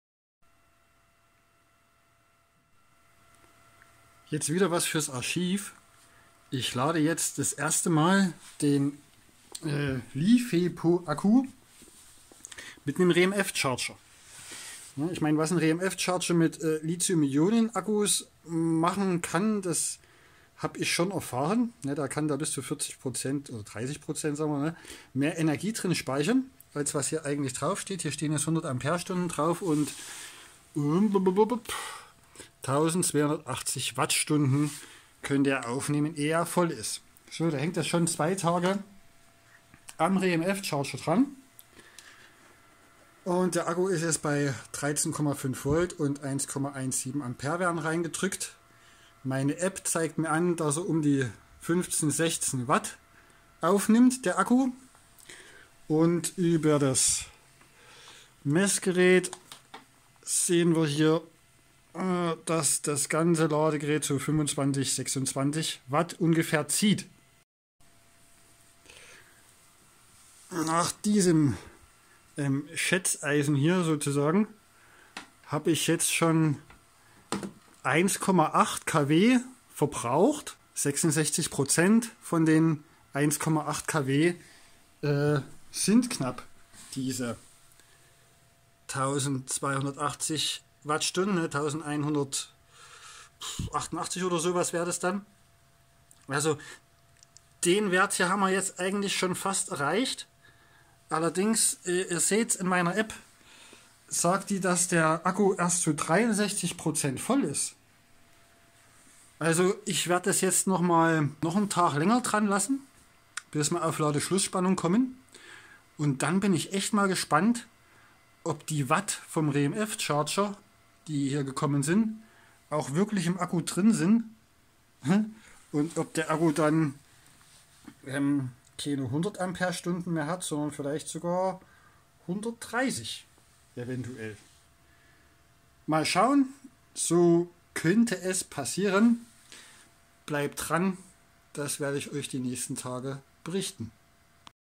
Vatrer LiFePo 100Ah Re-EMF Charger Test - 30% mehr Kapazität speichern als vom Hersteller angegeben?
Nun ist auch endlich der LiFePo Akku dran mit Re-EMF Charger geladen zu werden.
Ursprünglich vom Niederländer Rene Reijnders konzipiert für Ammonium Alaun Blei Akkus kann der Re-EMF Charger auch Bleisäure Akkus, Ni-Mh, Ni-Cd und Li-Ion laden. Bei Ni-Mh und Li-Ion mittlerweile nachweislich mit mehr Kapazität als von Akku Hersteller angegeben. Bis zu 40% Kapazitätssteigerung bei Li-Ion und 70% bei Ni-Mh Akkus wurden bisher dokumentiert.
Fehlt noch der Test mit LiFePo Akku welcher jetzt getätigt wird.